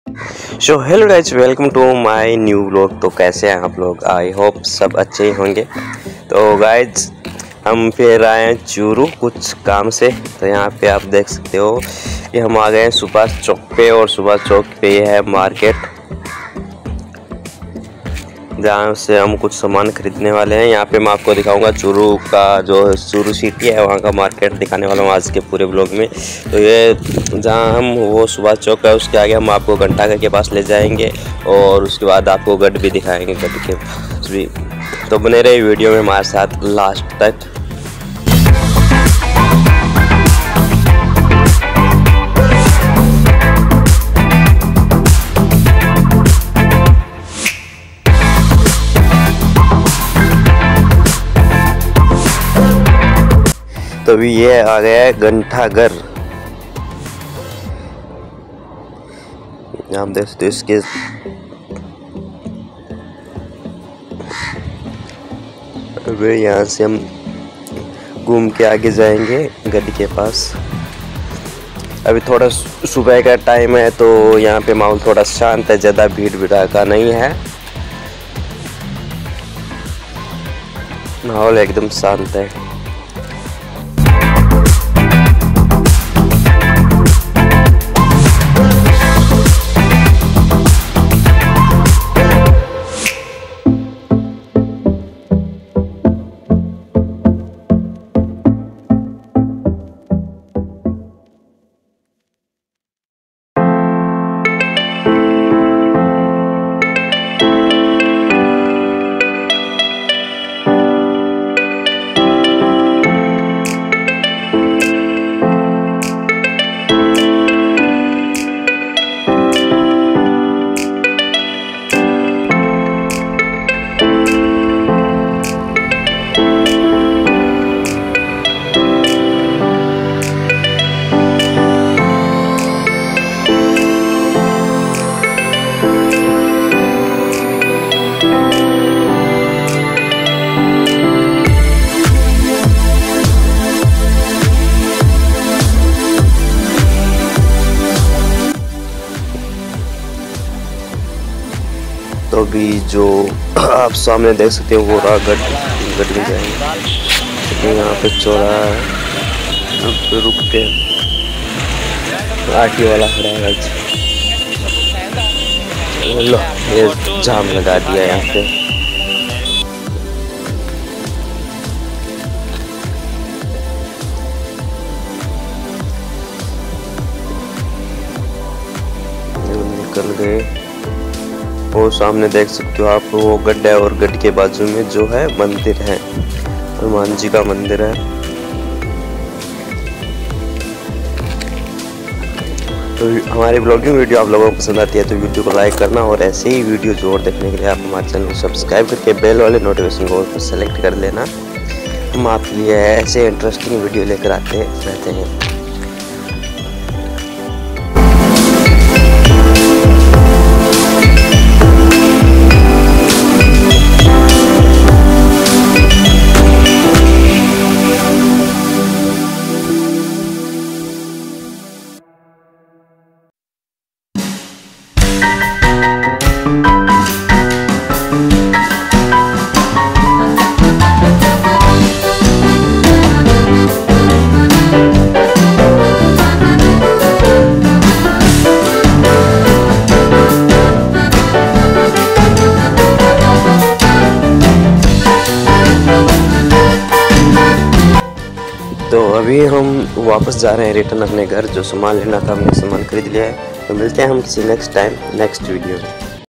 CHURU CITY | आज आपको चुरु शहर में घुमाते हैं | Rajasthani Vlogs
Second channel :- Omprakash Hooda